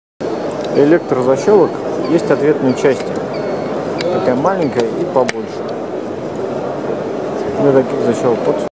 Smartec ST-SL120L Ответная часть для электро защелки
MIPS 2016. Smartec ST-SL120L
Один из вариантов ответной части для электро-защелок. Можно купить в нашем интернет-магазине. BestSB.ru перейдя